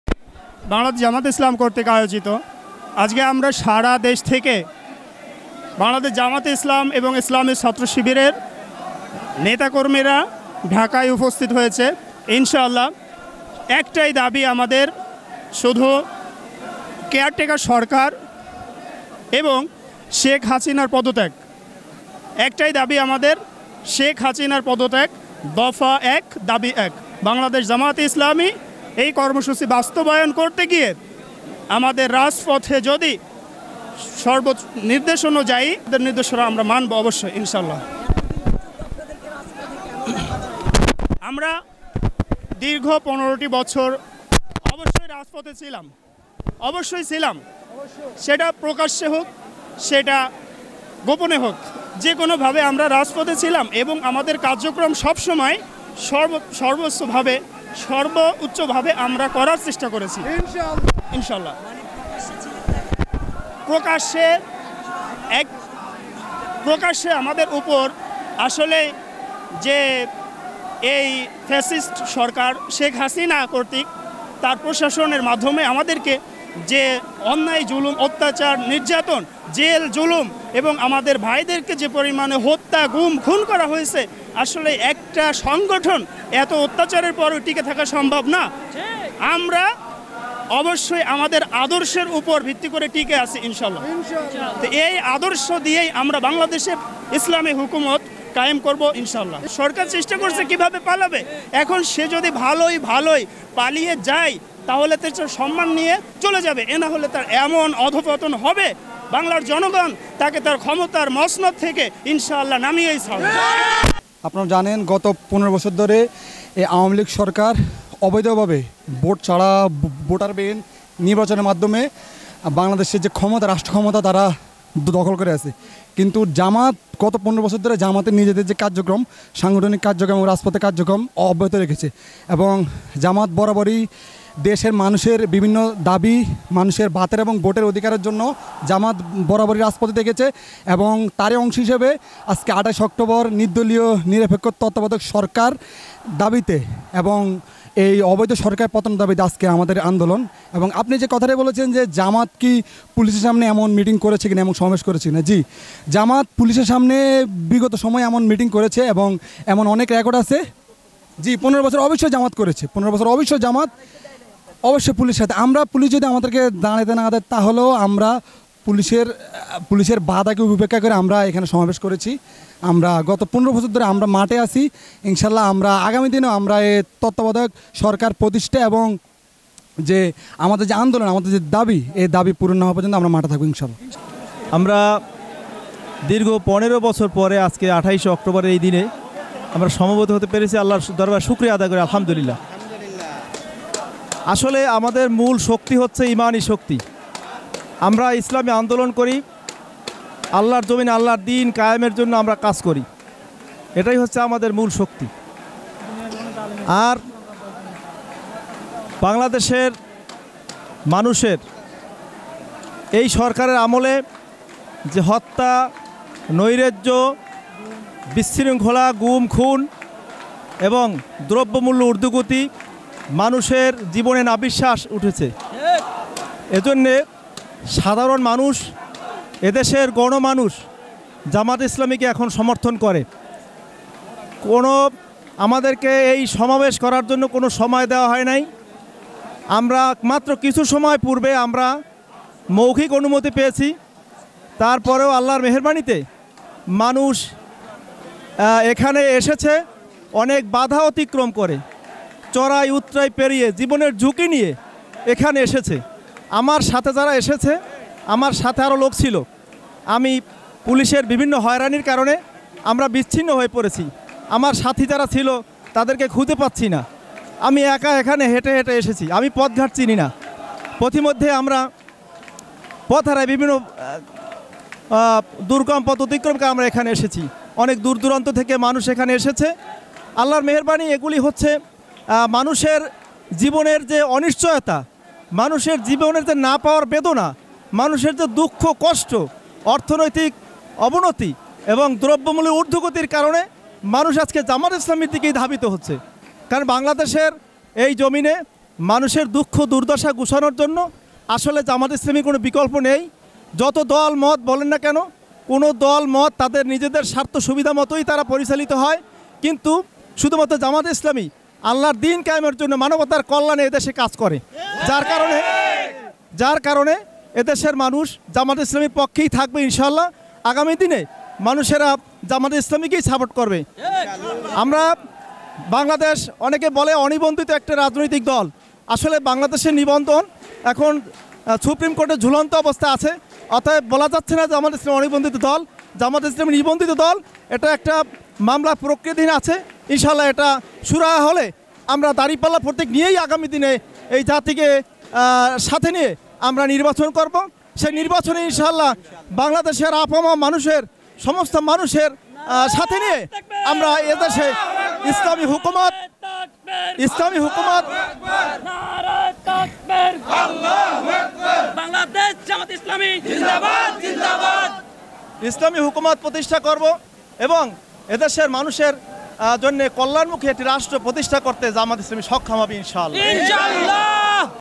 ২৮ অক্টোবর জামায়াত শিবিরের তৃণমূল কর্মীরা যা বললেন।
২৮ অক্টোবর ২০২৩ বিএনপি ঘোষিত মহাসমাবেশের মতিঝিলে সমাবেশ করে জামায়াত। এসময় তৃণমূল নেতাকর্মীরা প্রতিক্রিয়া দেন।